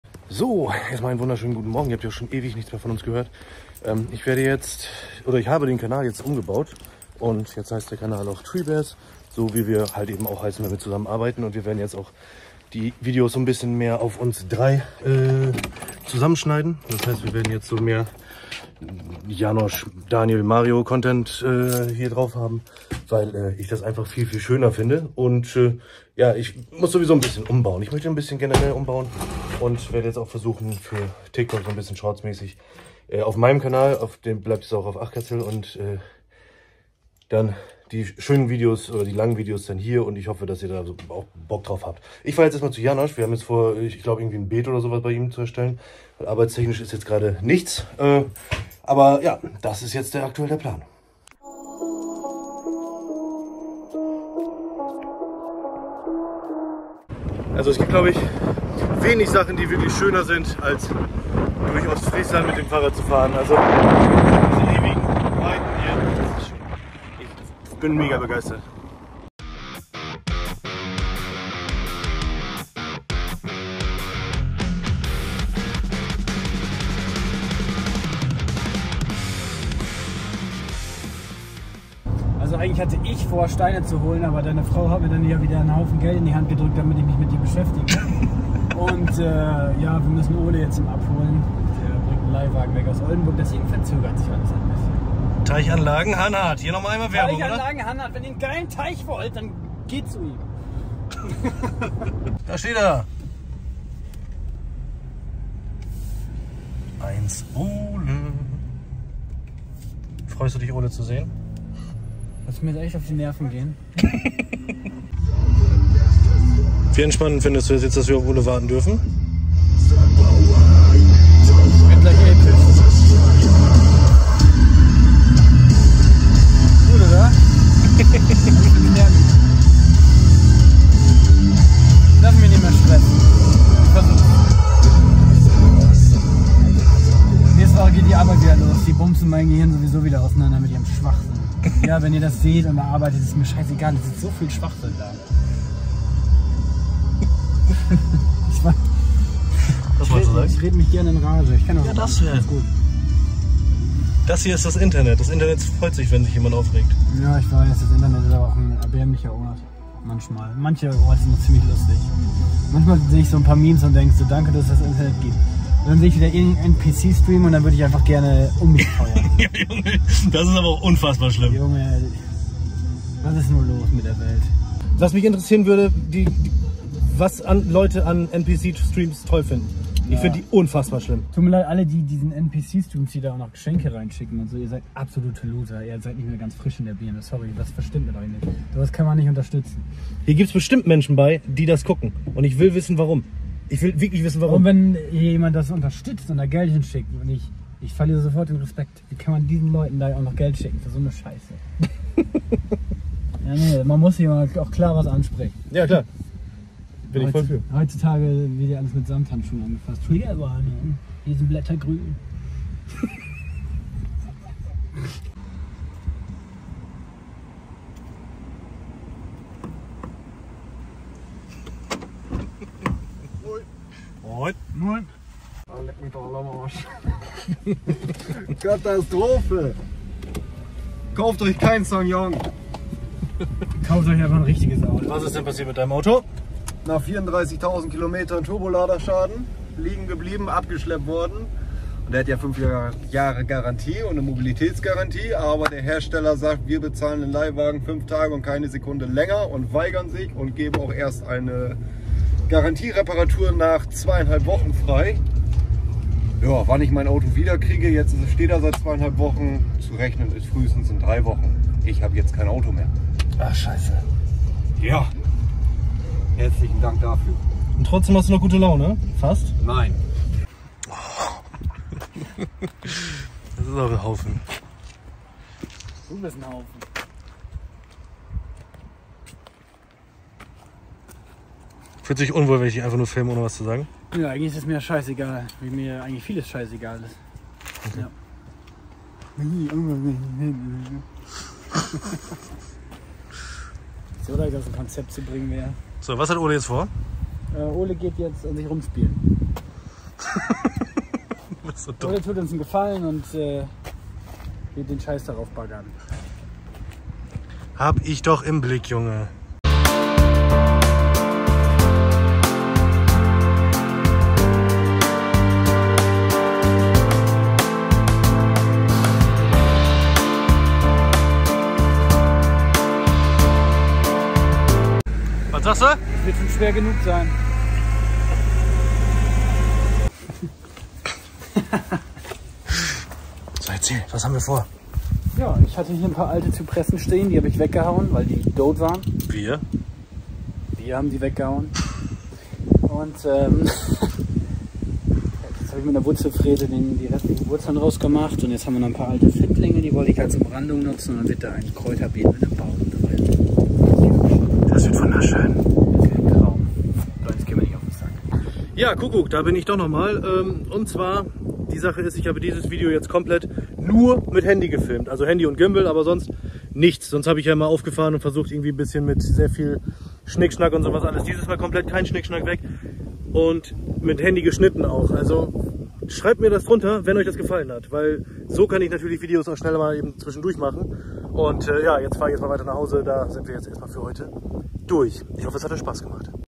Treebears unterwegs: Von Beetgestaltung zu spontanen Abenteuern: Ein Tag mit Janosch & Mario
Ein Tag voller Überraschungen mit Janosch und mir, Mario. Dieses mal etwas abseits der Baumpflege. Was als einfacher Plan begann, ein Beet bei Janosch zu gestalten, wurde zu einem spontanen Abholtrip nach Oldenburg um Ole von Teichanlagen Hanhardt abzuholen. Und wie das Schicksal es wollte, konnten wir am Ende des Tages doch noch die nötigen Steine für Janoschs Beet auftreiben. Begleitet uns auf dieser unerwarteten Reise und erlebt mit, wie manchmal die besten Geschichten einfach passieren, wenn man am wenigsten damit rechnet.